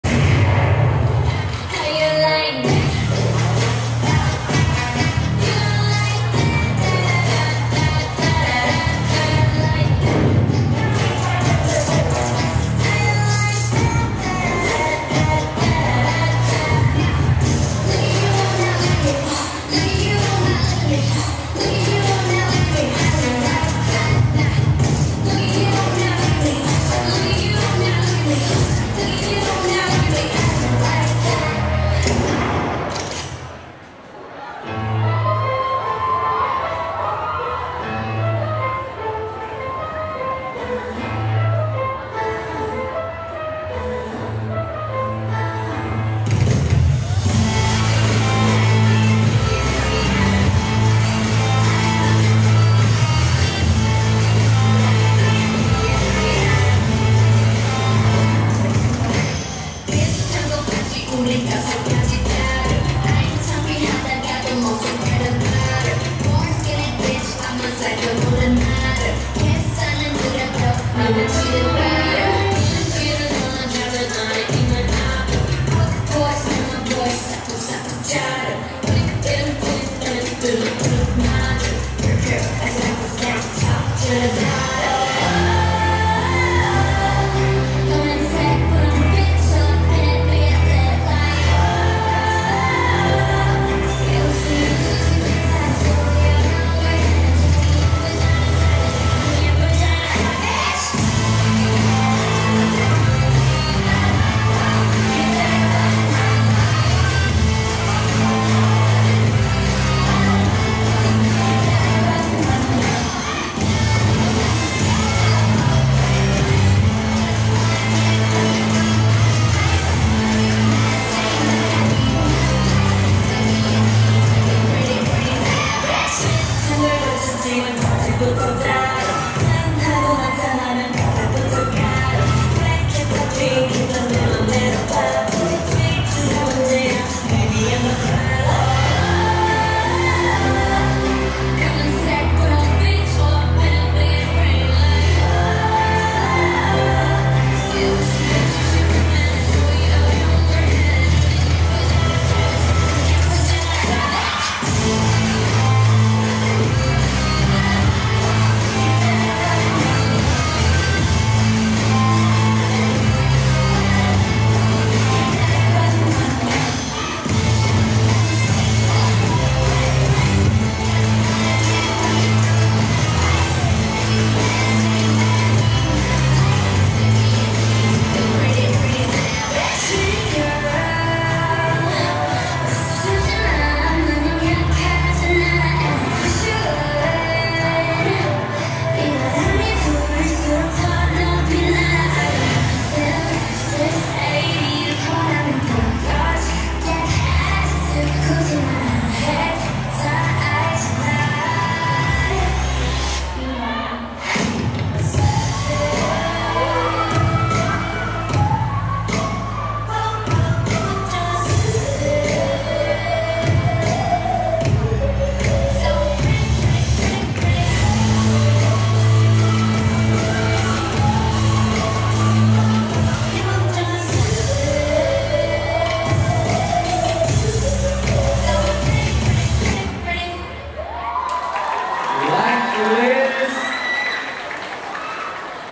210327 BLACKLIST cover BLACKPINK​ [Full Cam] │@MBK Happy Summer Cover Dance 2021
@MBK Happy Summer Cover Dance 2021 By DP Dance Studio  (Thailand)

ⓒ 2021 GBewStudio.official